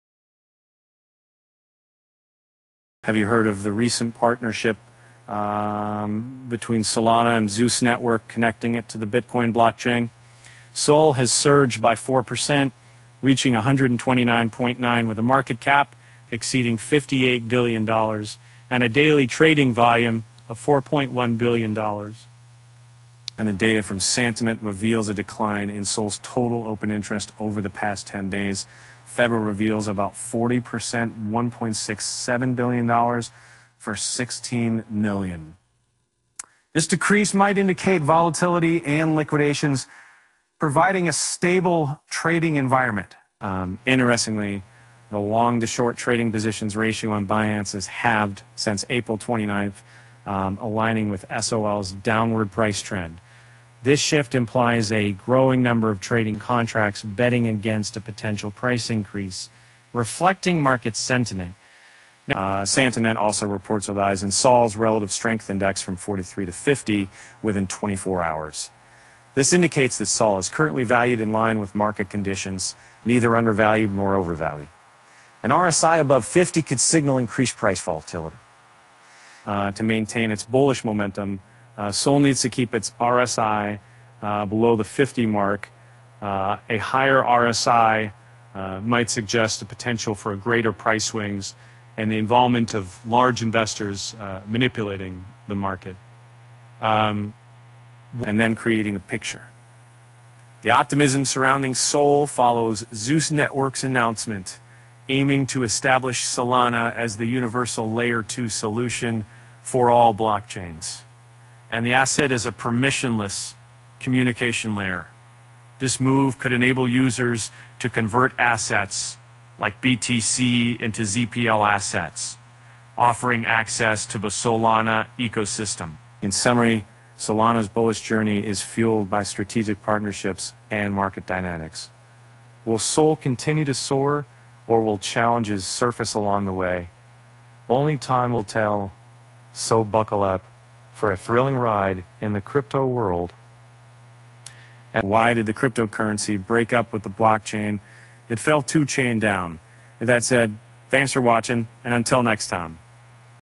Solana gains momentum following Solana-Bitcoin cross-chain announcement
Have you heard of the recent partnership between Solana and Zeus Network connecting it to the Bitcoin blockchain? SOL has surged by 4%, reaching $129.9 with a market cap exceeding $58 billion and a daily trading volume of $4.1 billion.